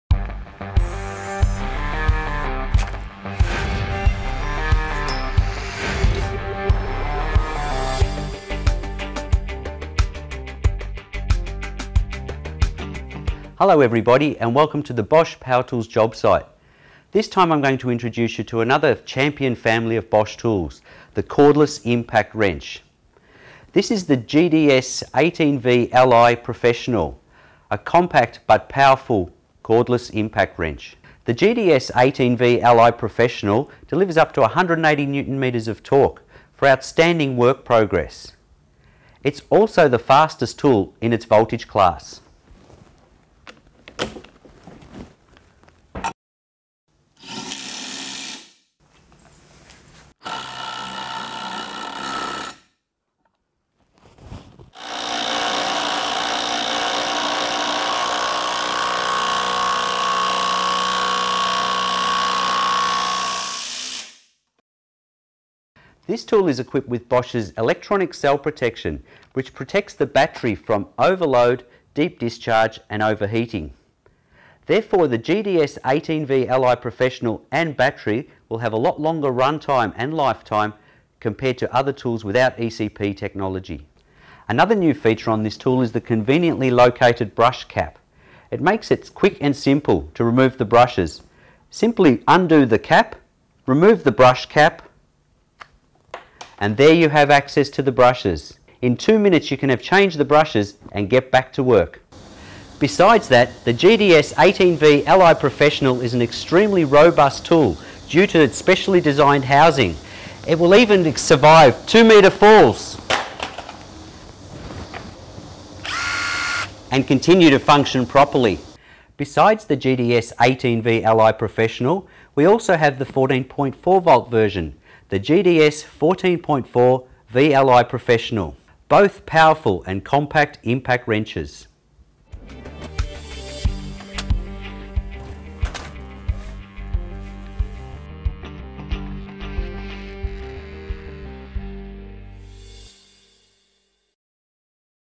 Impact Wrench | Impact Screwdriver | GDS 14,4 V-LI | GDS 18 V-LI
Bosch GDS 14, 4/ 18 V-LI Professional Cordless Impact Wrenches is a powerful, compact and handy impact wrench. This Bosch drill tool has the highest power and highest speed in its class of tools for an outstanding work progress. The compact and ergonomic grip design of the power tool enriches the user with a fatigue-free working and its optimized head length is good for working in a narrow space. It provides the unique choice between “premium” (longer battery runtime) and “compact” (less weight for even more comfortable work) batteries. It consists of a high torque and high impact rate (3,200bpm) for fast work progress when driving screws in metal (up to M 16) and wood. It has a very short ergonomic design for perfect handling, even in the tightest spaces and overhead. It includes a unique Bosh premium lithium-ion technology for an unbeatable battery runtime. The Bosch Electronic Cell Protection (ECP) protects the battery against overload, overheating and deep discharge.